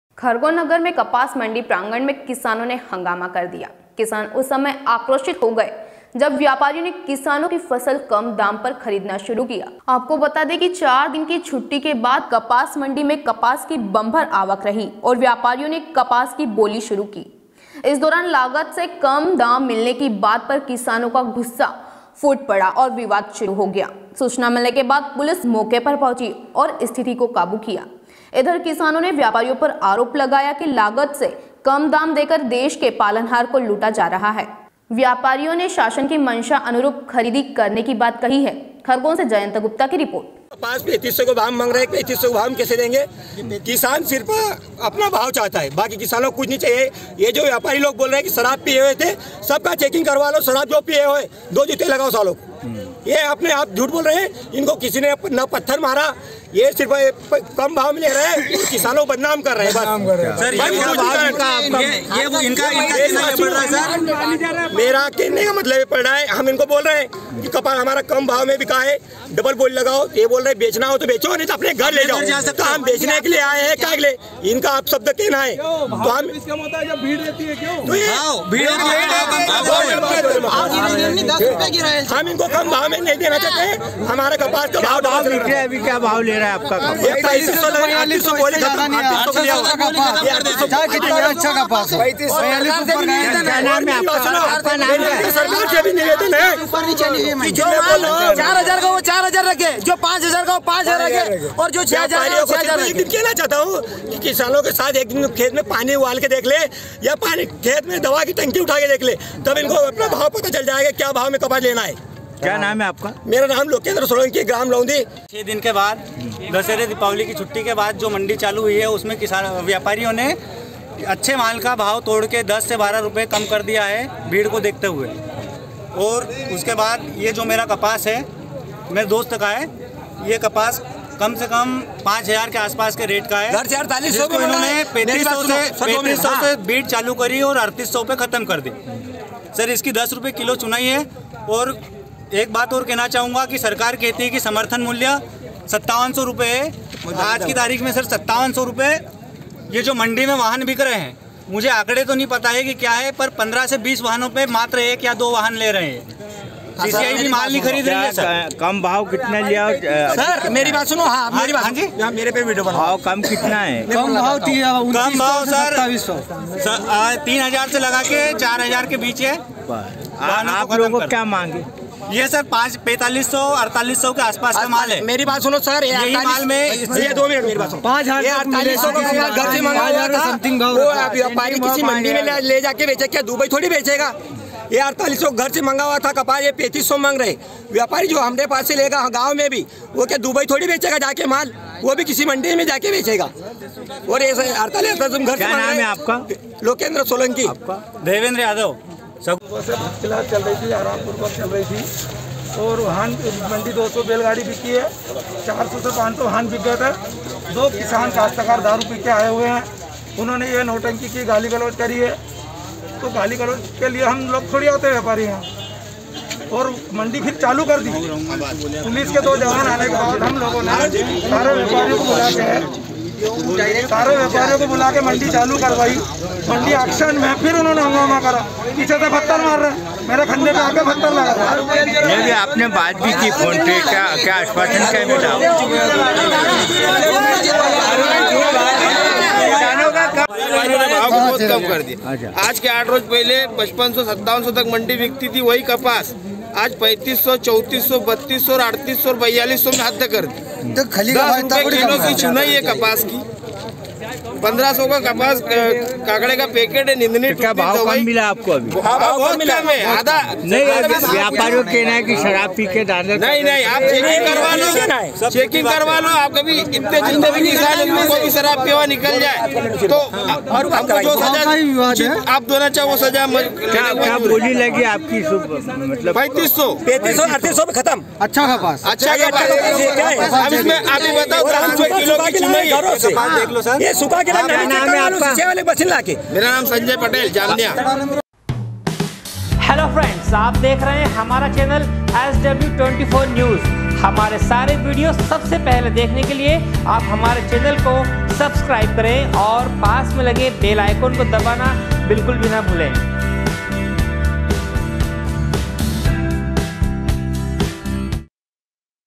Khargone | SW 24 News Madhyapradesh
खरगोन नगर में कपास मंडी प्रांगण में किसानों ने हंगामा कर दिया। किसान उस समय आक्रोषित हो गए जब व्यापारियों ने किसानों की फसल कम दाम पर खरीदना शुरू किया। खरगोन से जयंत गुप्ता की रिपोर्ट